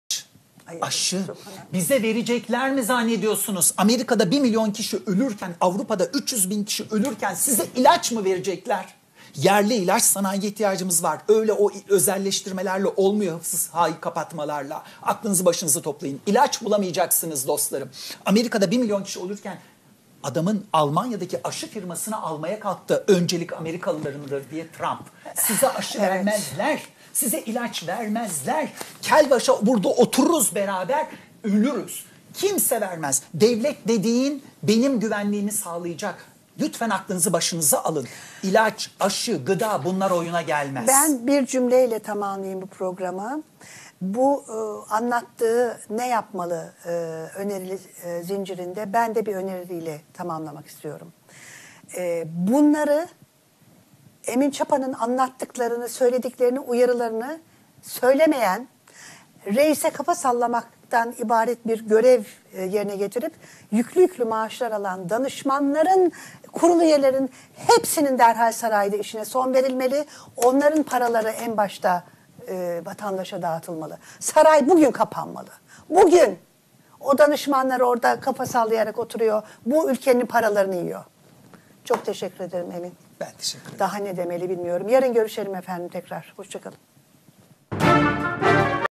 Saray derhal kapatılmalı - Paralar insana dağıtılmalı
Cok önemli sözler. Tarihe gececek sözler.
Amerika Avrupa kimse Türkiyeye yardım etmiyecek. Kendimis üretmeye başlamalıyız. Saray derhal kapatılmalı. Paralar insana dağıtılmalı
Tümü izlenir